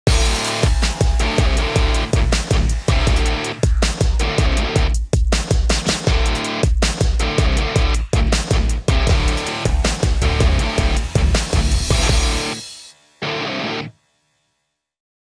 WLMMA 46 München: Jakub Marek vs Laimonas Mockus
Weltergewicht – das steht für Knockout Power und Speed. Diese beiden Skills zeigen Marek vom renommierten Guardian Gym und Mockus, welcher unter der Aufsicht von UFC-Legende Dennis Siver gedeiht, per excellence im Octagon.